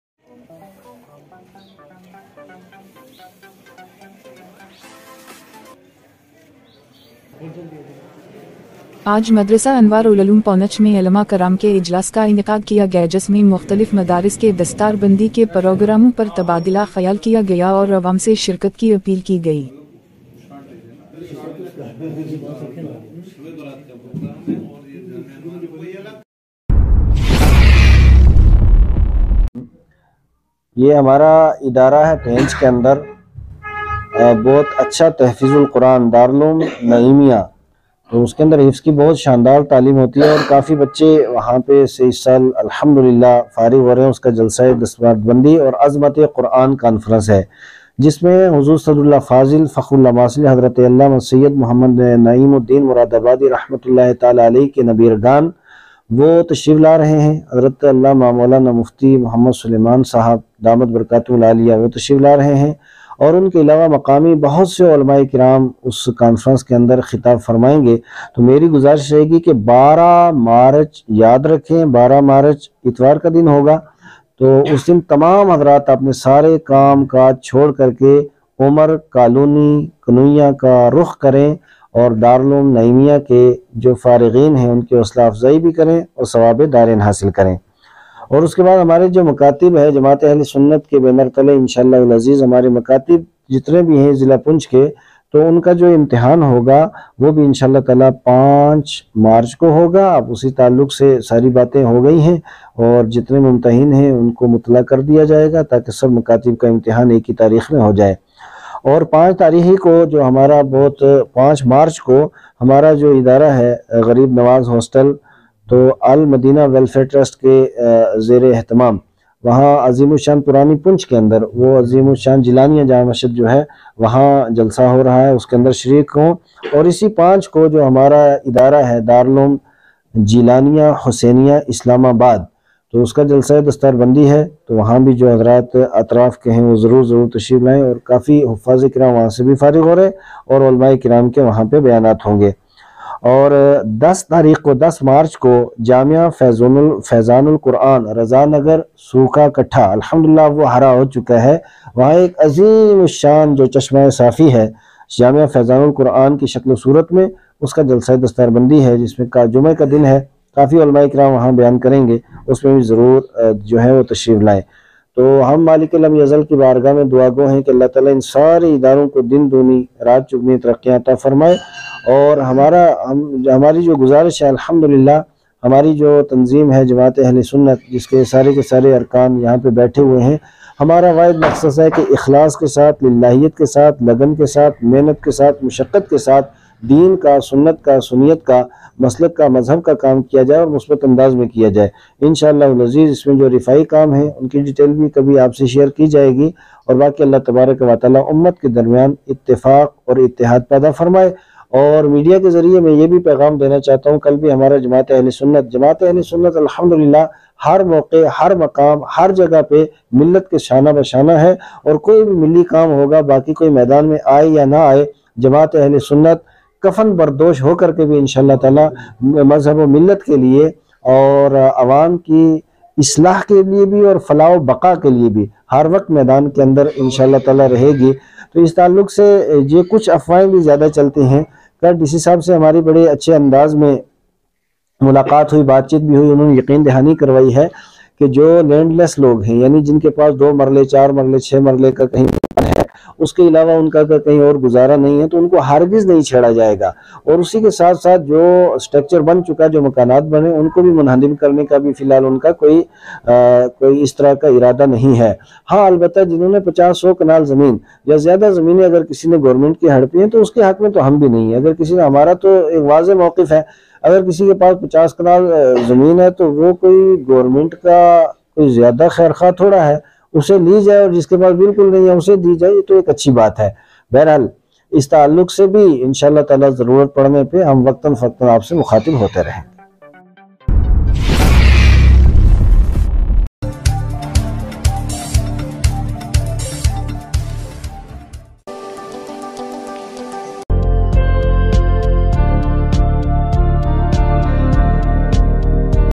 آج مدرسہ انوار العلوم پونچھ میں علماء کرام کے اجلاس کا انعقاد کیا گیاجس میں مختلف مدارس کے دستاربندی
Waqt ki Awaaz Jk News

آج مدرسہ انوار العلوم پونچھ میں علماء کرام کے اجلاس کا انعقاد کیا گیاجس میں مختلف مدارس کے دستاربندی کے پروگراموں پر تبادلہ خیال کیا گیا اور عوام سے شرکت کی اپیل کی گئی۔